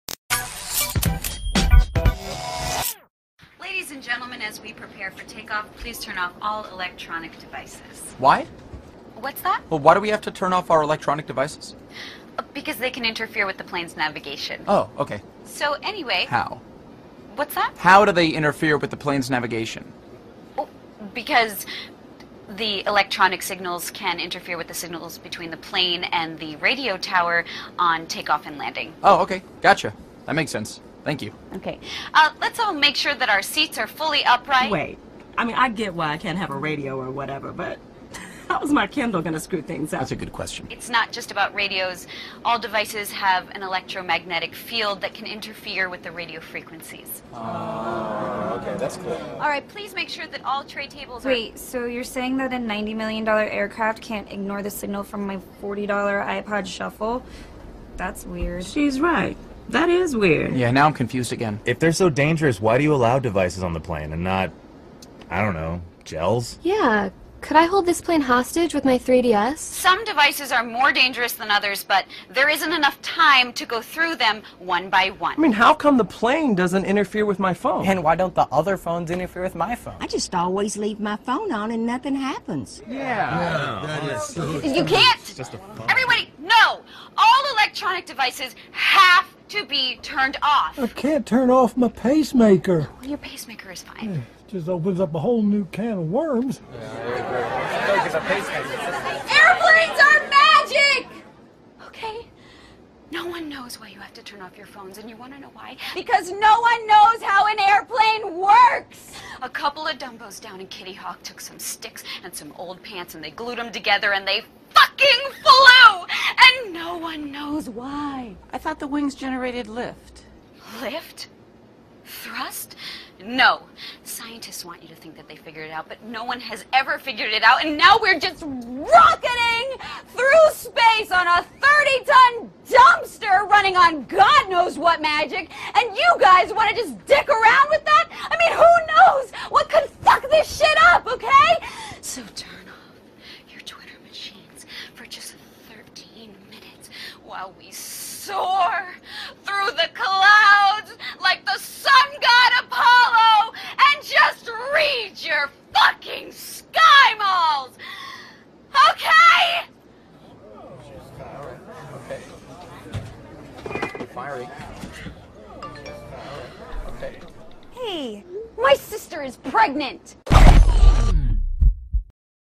Neden uçakta telefon kullanamayız?
Hostes yolculara telefon vb. elektronik aletlerini uçağın kalkış ve iniş sırasında kapatmalarını söylüyor. Yolculardan biri nedeni soruyor. Veee...